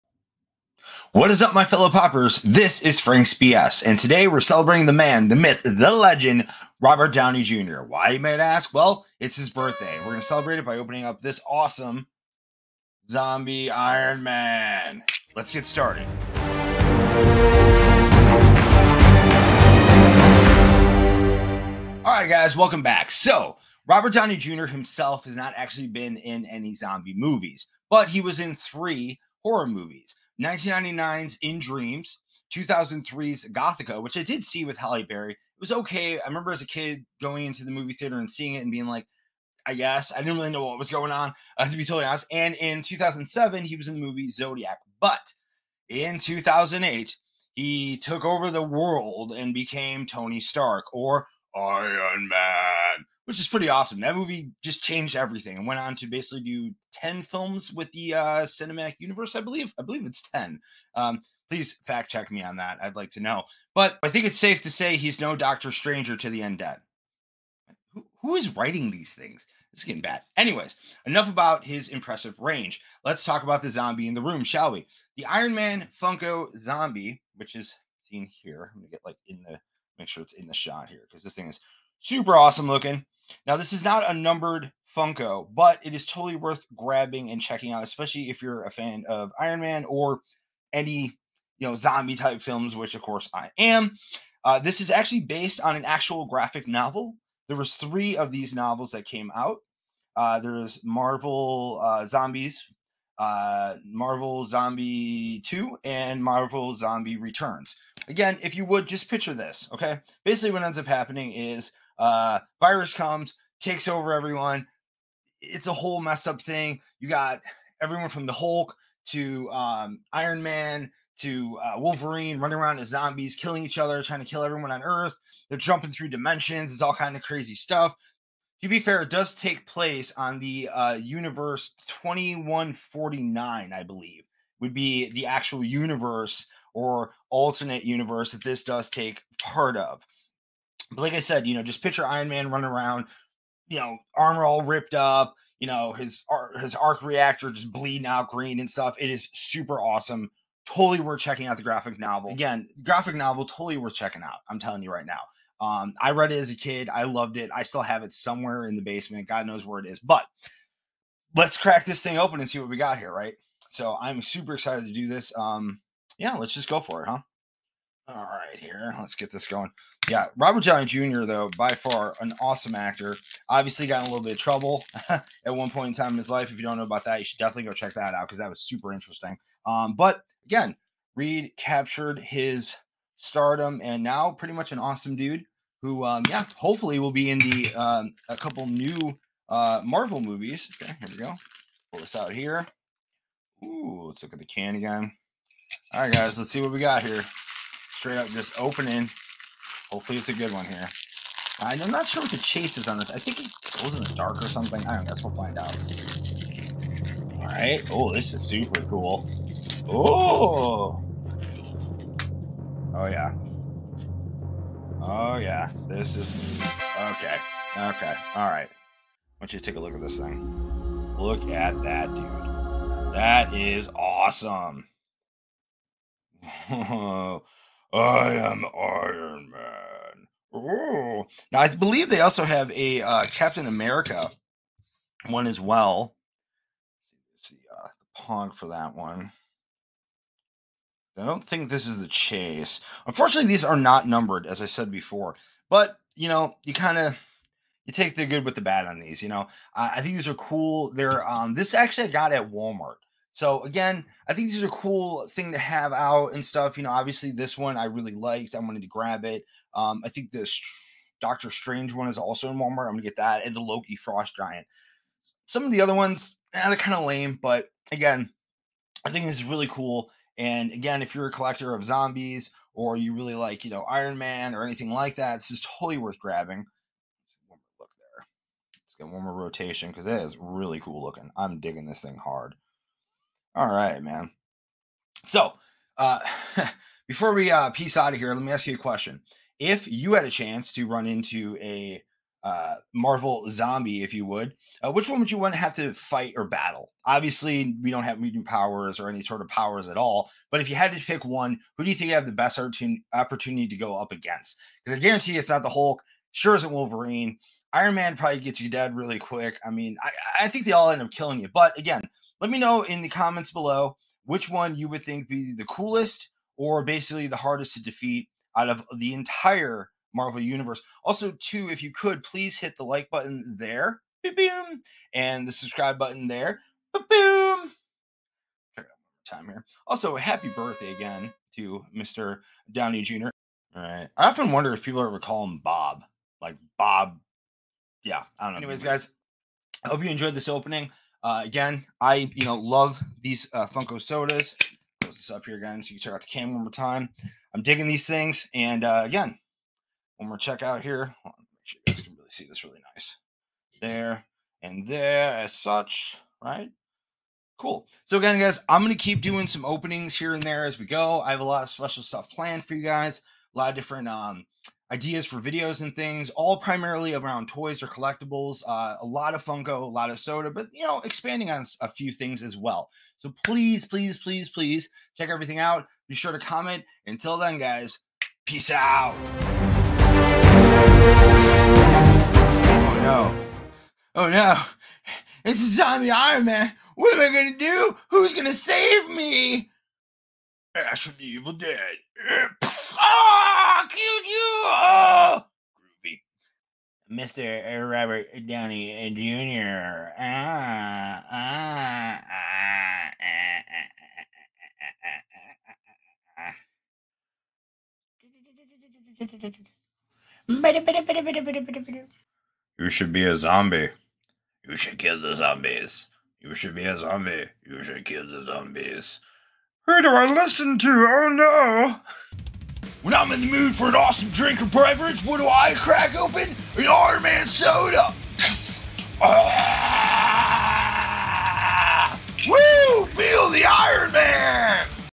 Zombie Iron Man Funko Soda Unboxing: A Birthday Gift for Robert Downey Jr. and Marvel Fans Alike
Happy birthday to Robert Downey Jr., the man behind Marvel's iconic superhero Iron Man! To celebrate the occasion, we're unboxing the limited edition Zombie Iron Man Funko Soda, which features the undead version of Tony Stark. This is a must-have collectible for Marvel and Funko fans alike, and we couldn't resist sharing the unboxing experience with you. Join us as we unveil this spooky surprise and add it to our growing collection.